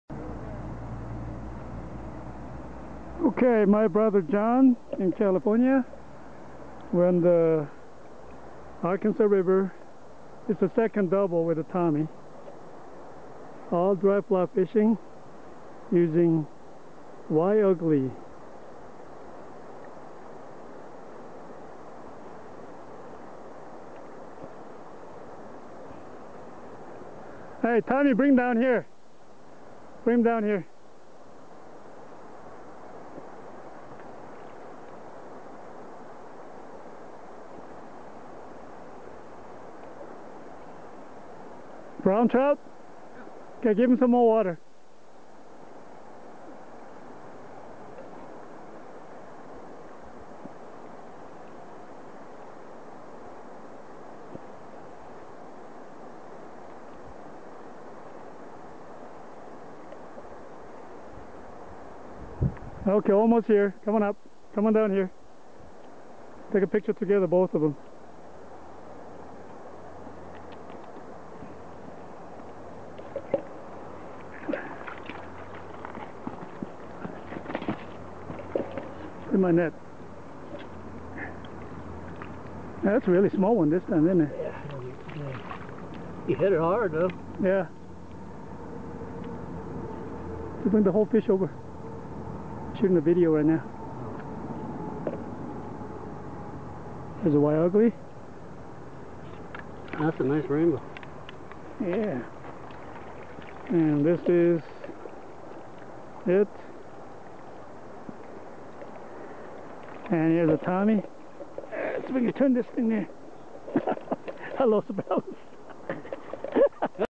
Caddis hatch Arkansas River Colorado dry Fly fishing with Y-Ugly fly
Tom and I had double few times on Arkansas River caddis dry fly fishing.
We used Y-Ugly fly and caught many trout. April 2009